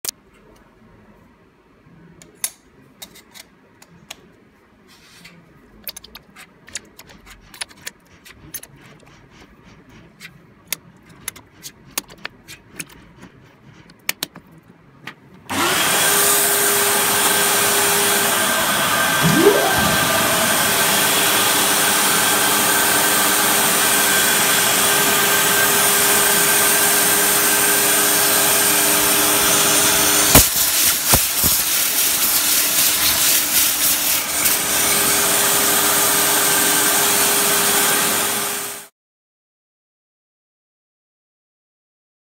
Satin finish small tag plates 1.2mm thick
Using vacuum tabe and skeleton frame.